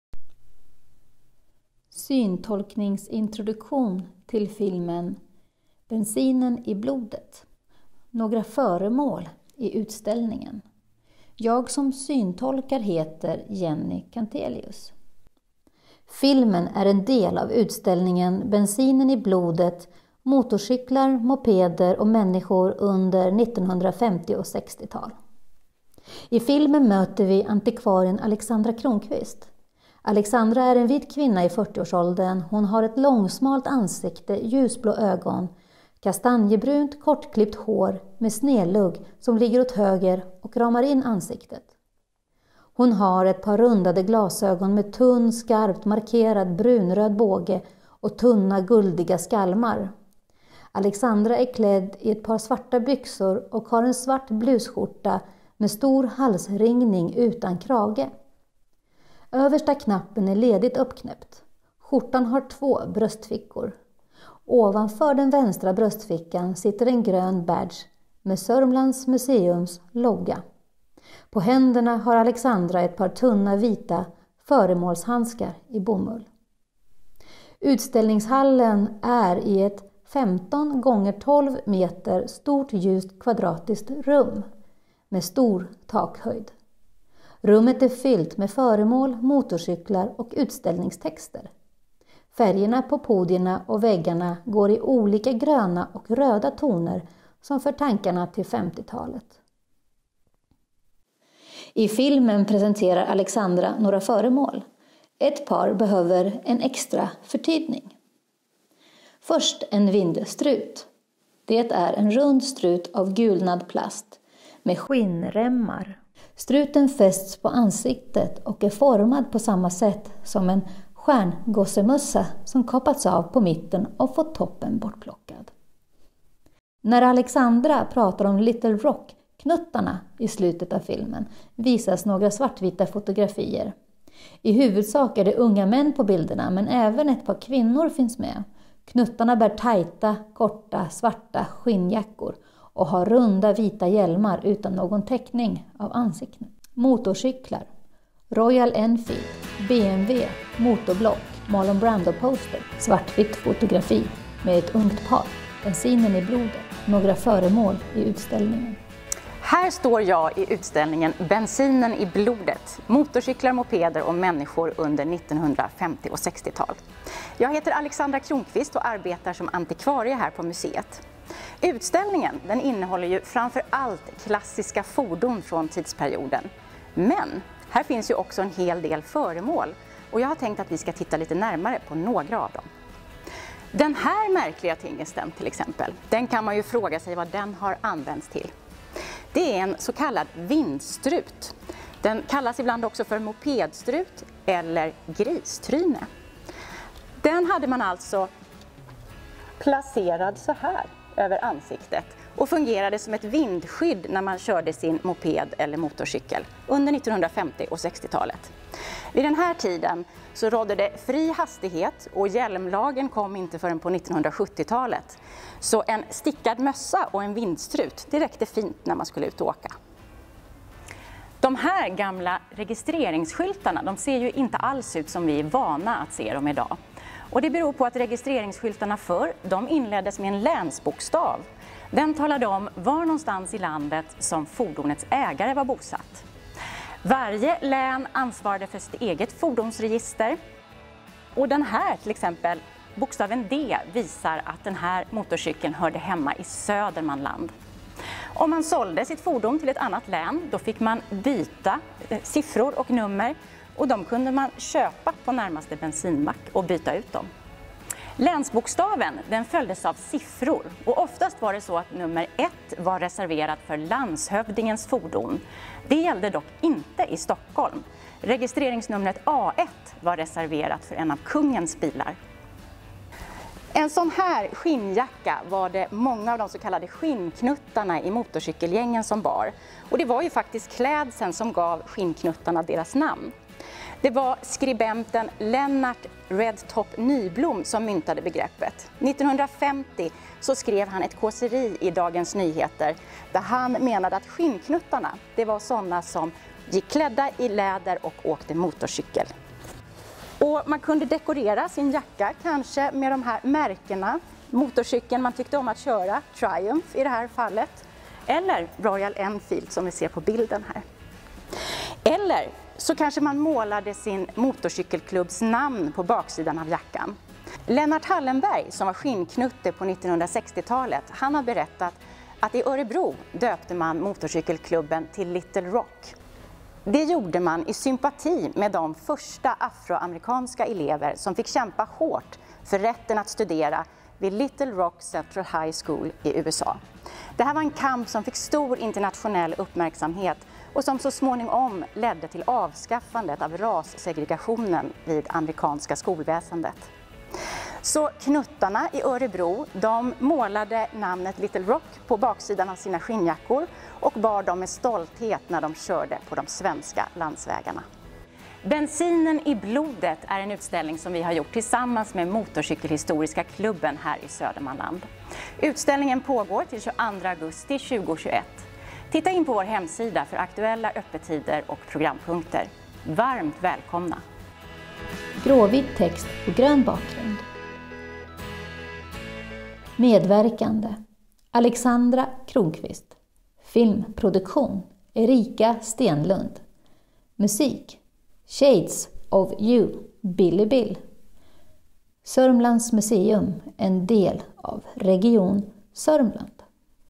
Bensinen i blodet! Några föremål i utställningen - Syntolkad
Alexandra Kronqvist antikvarie på Sörmlands museum berättar om några intressanta föremål som ställs ut i utställningen Bensinen i blodet! Motorcyklar mopeder och människor under 1950 och 60-tal. En vindstrut, om tidens registreringsskyltar och den identitetsbyggande  läderjackan.

Utställningen pågår till 22 augusti 2021 och är ett samarbete med Motorcykelhistoriska klubben i Södermanland.